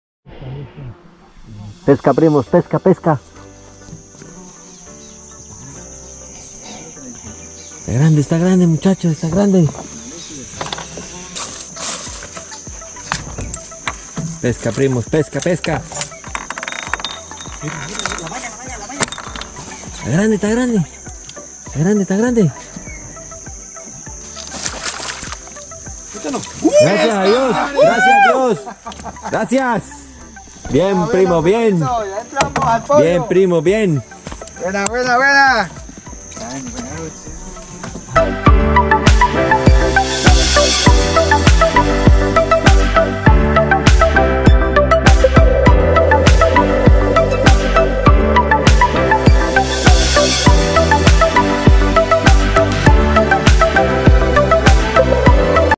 PESCA DE TUCUNARE
pesca de tucunare en una represa de  Colombia
muy buena pesca